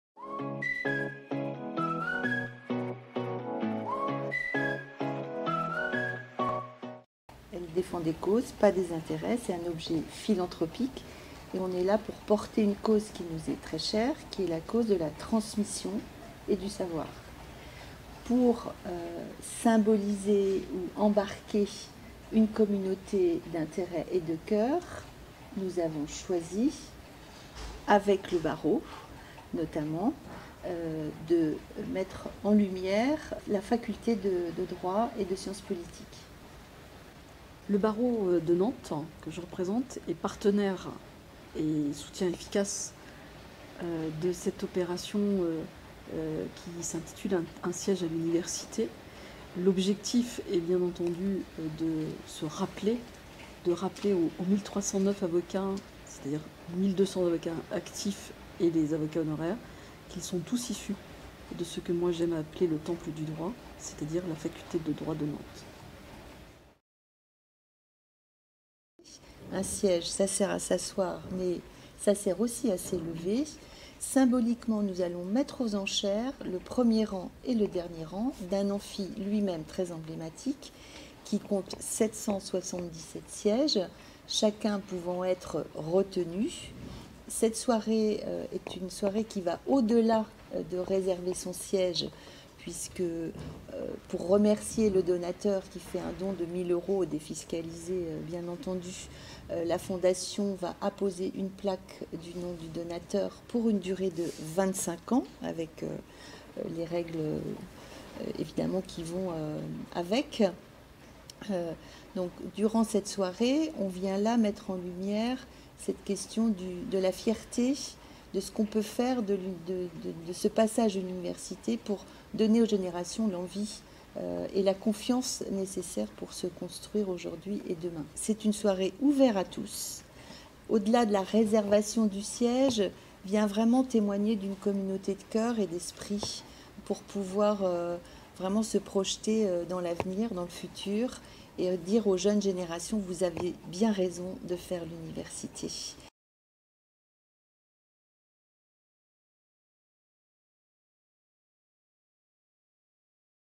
Un siège à l'Université - Défendez l'avenir du droit
Un siège, ça sert à s'asseoir mais aussi à s'élever !

Montrez votre attachement à la faculté de droit de Nantes en inscrivant votre nom dans l'amphithéâtre.

La Fondation Nantes Université  lance l'opération "Un siège à l'Université" et invite tous ceux qui ont fait l'histoire de la Faculté de Droit et des Sciences politiques à valoriser les métiers du droit.

Par vos dons, vus contribuez au rayonnement de la faculté de droit et offrez à nos étudiants et futurs professionnels un cadre d'apprentissage de qualité. Pour que, toujours, l'Université de Nantes forme nos juristes de demain, égaux, libres et indépendants.

Labellisée Patrimoine remarquable du XXe siècle, la Faculté de Droit et des Sciences Politiques de Nantes forme chaque année près de 4300 étudiants dont 80% restent et pratiquent le droit en Pays de la Loire. Construite dans les années 70 par l'architecte Louis Arretche, la faculté possède l'amphithéâtre le plus grand mais aussi le plus emblématique du campus : l'amphithéâtre E. Les plus anciens se souviendront de son rideau métallique...